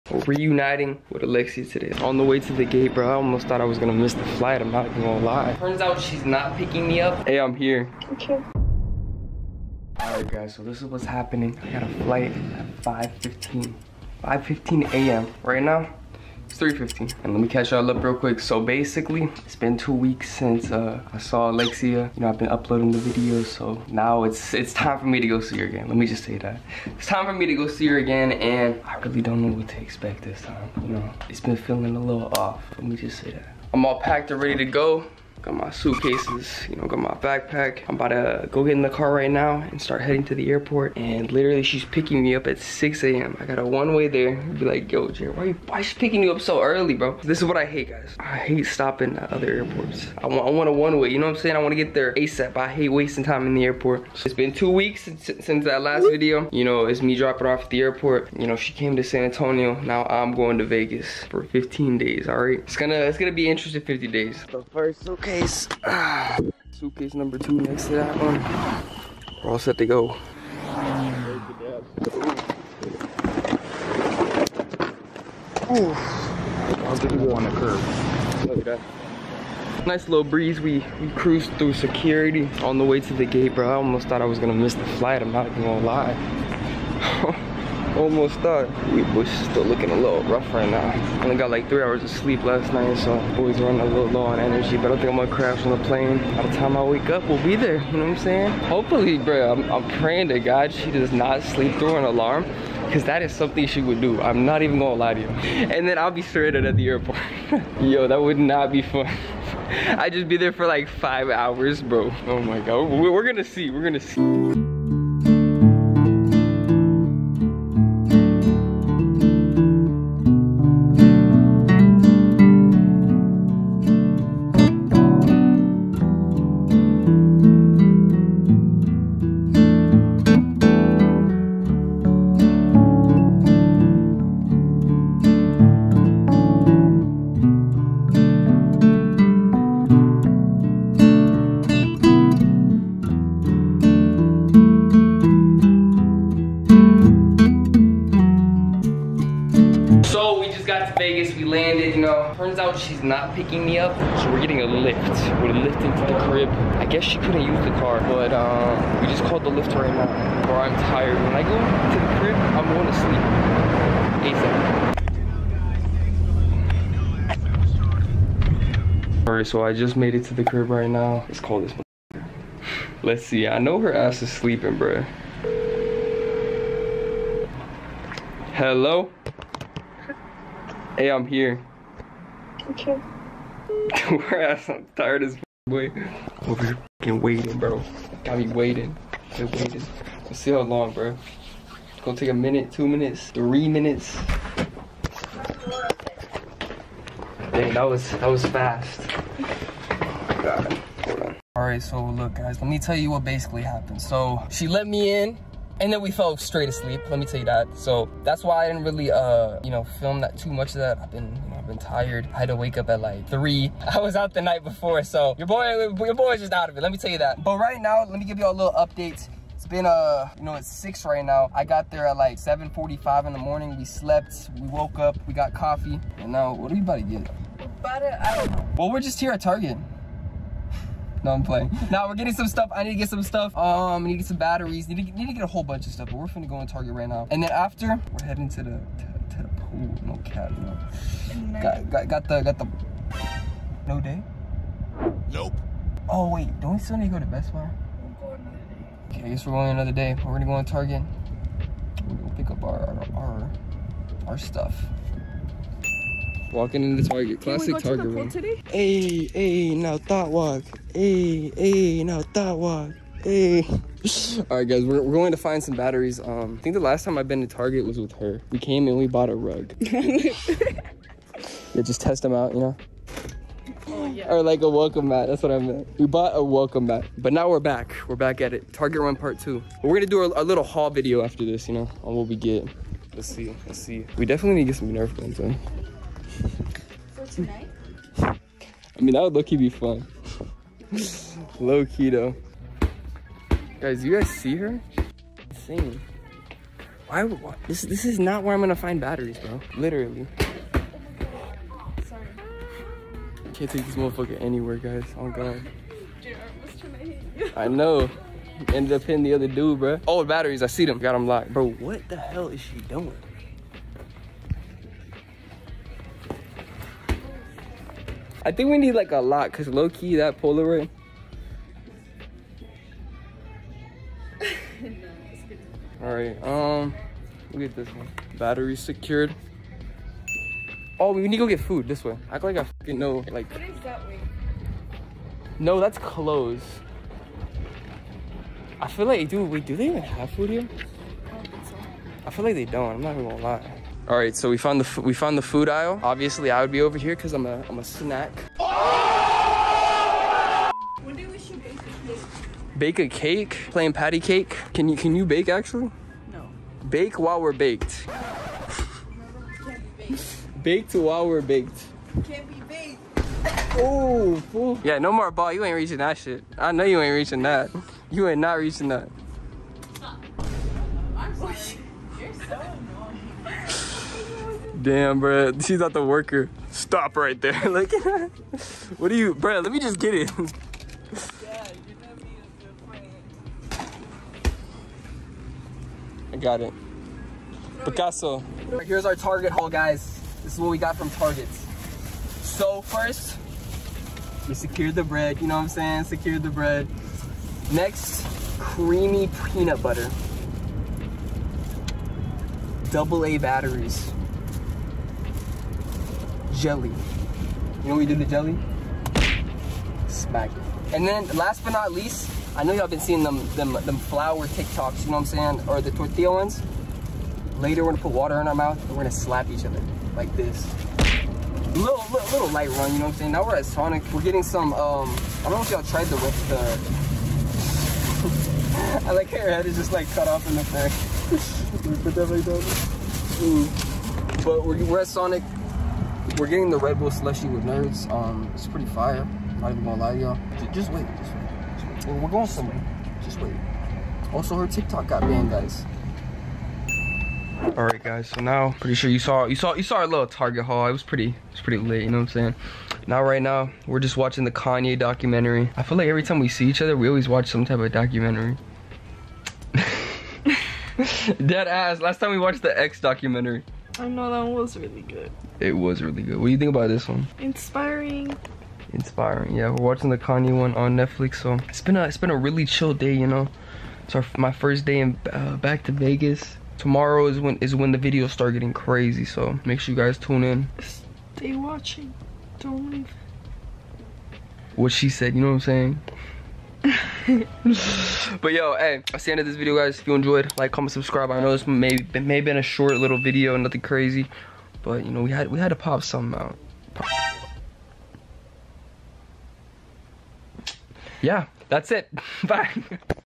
I Missed Her! So I Flew To Vegas
Here Me reuniting with Alexia!

Go Get You Some ;)